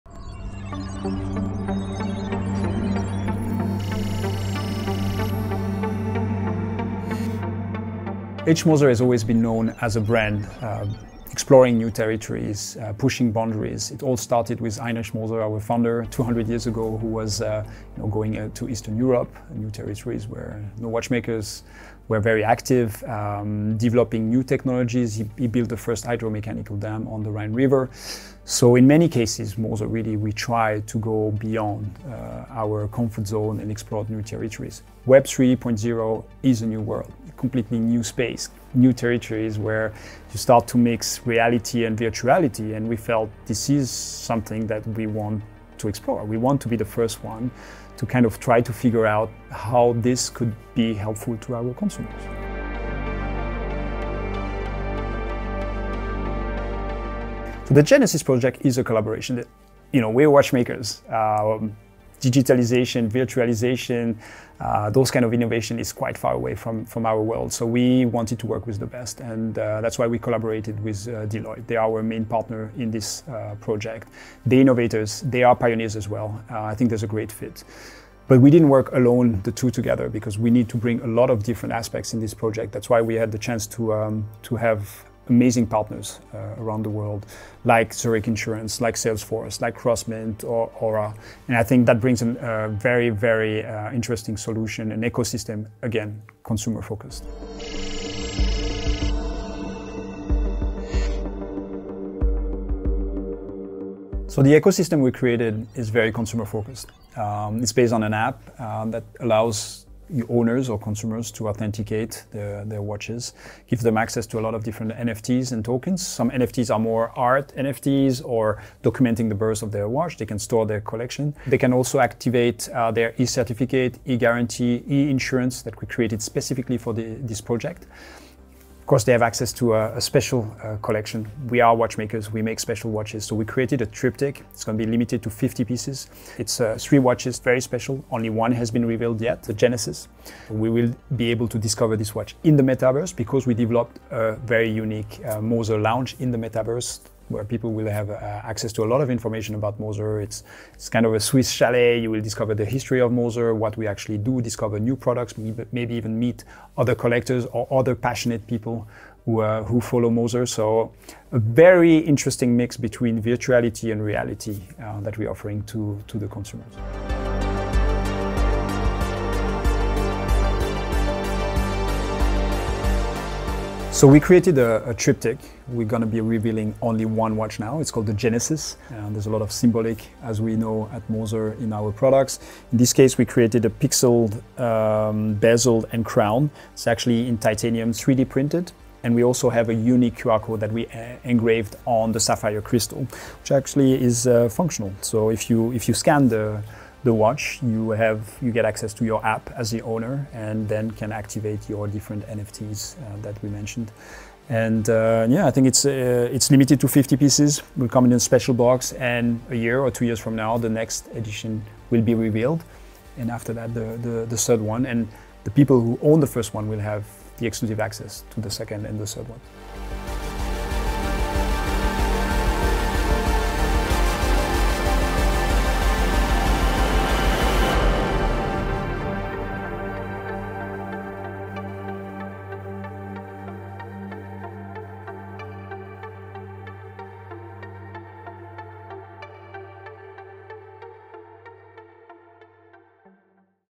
H. Moser & Cie - The GENESIS Project
Keeping up with the times, because even an industry based on tradition must innovate to create the best possible customer experience. H. Moser & Cie. is diving into a new world of possibilities, with the aim of offering a new immersive luxury experience for its customers with the Genesis project.

Using an approach that combines physical, digital and virtual dimensions, H. Moser & Cie. is introducing a cutting-edge, customer-focused holistic ecosystem using decentralized technologies, including blockchain watch authentication, digital assets and its own dedicated metaverse space. H. Moser & Cie. has embarked with select partners including Deloitte Switzerland, Aura Blockchain Consortium, Zurich Switzerland, Crossmint and Salesforce, all leaders in their respective fields who have come together with H. Moser & Cie.  to utilise new  technologies that enable enhanced security and deeper user engagement.

This pioneering project is being piloted with a concept watch available as a limited edition of 50 pieces: the Endeavour Centre Seconds Genesis. Blurring the lines between virtual and tangible, its 3D-printed titanium crown and bezel appear pixelated and are perfectly paired with the watch's microblasted steel case. This is the first piece in an exclusive triptych which will give each owner access to the next two pieces.

Each Genesis timepiece bears a unique QR code engraved on its sapphire crystal that allows to authenticate watch ownership through a dedicated Moser application. It allows the owners to access to the entire dedicated digital and virtual ecosystem.